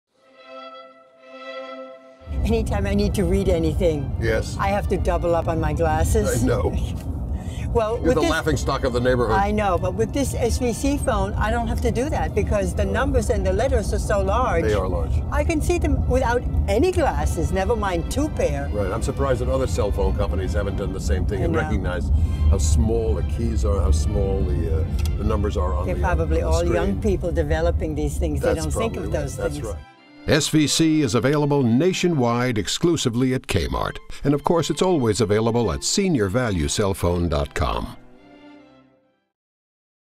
SVC phones are eye-friendly!
SVC is great for seniors because of how easy they are to see.